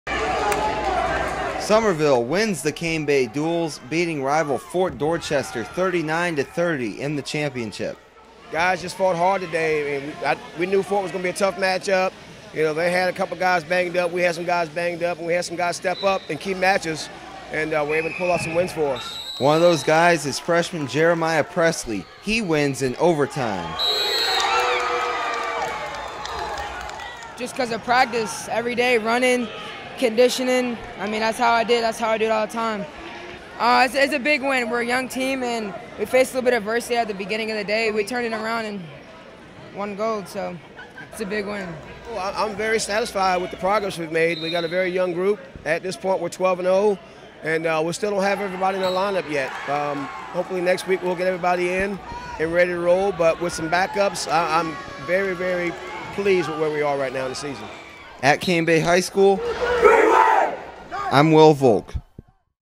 Summerville wins the Cane Bay Duals
Summerville beats Fort Dorchester to win the Cane Bay Duals.